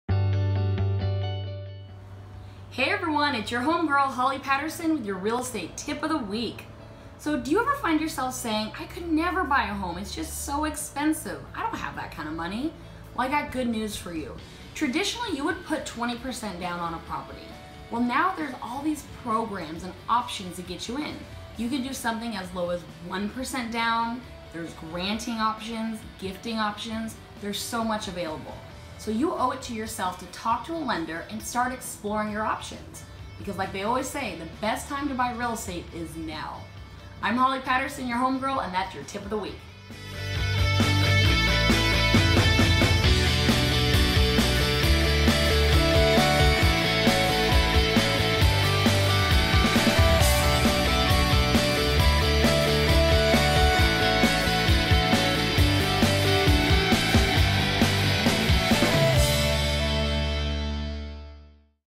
Holly Patterson's Real Estate Tip of the Week #1 - Down Payment Options
Real Estate Tip Of The Week -- Did you know you can buy a home with only having 1% for a down payment, not the traditional 20%?  Watch for more details!